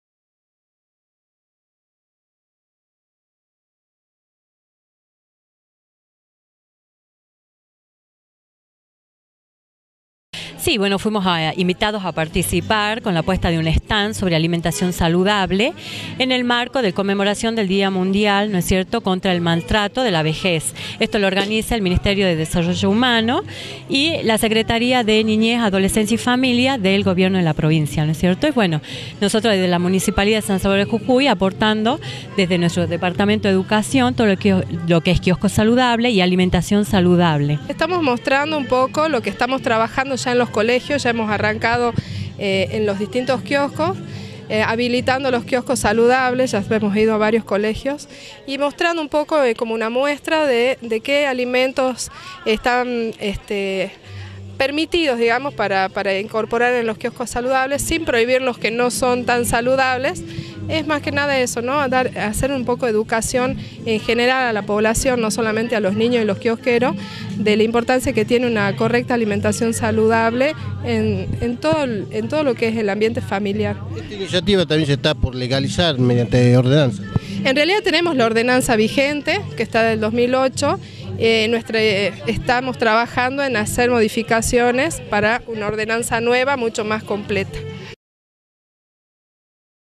Kiosco Saludables
en el marco del día mundial contra el maltrato a la vejez, la Direccion de Seguridad Alimentaria del municipio capitalino expuso un stand con información sobre alimento y kiosco saludable.